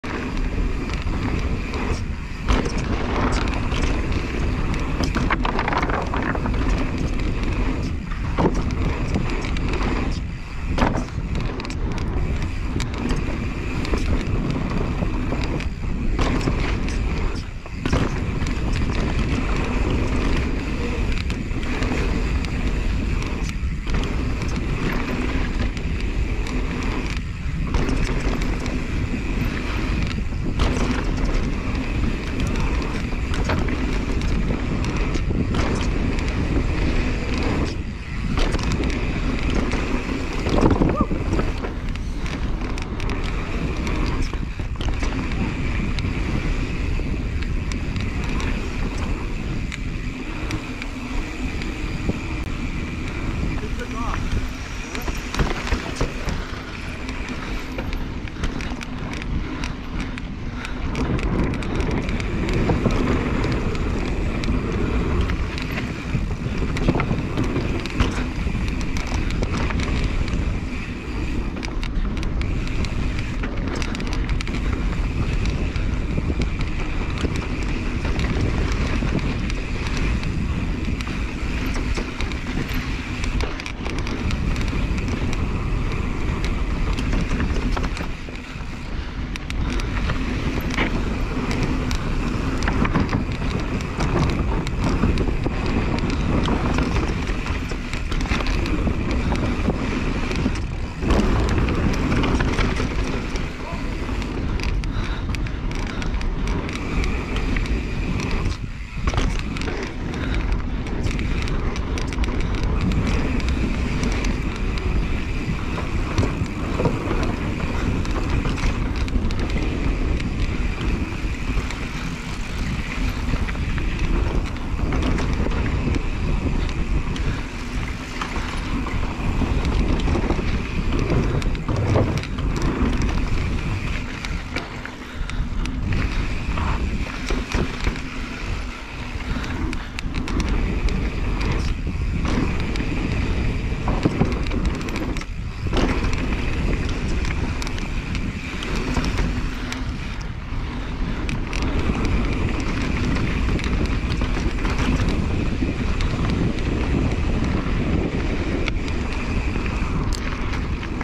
Rocky Mountain Element Skypark Bikefest 2022
Chasing Alvis down feature filled Arrow at Skypark Bike Fest
Demoing the Element C70 while following Alvis (Intense Tazer)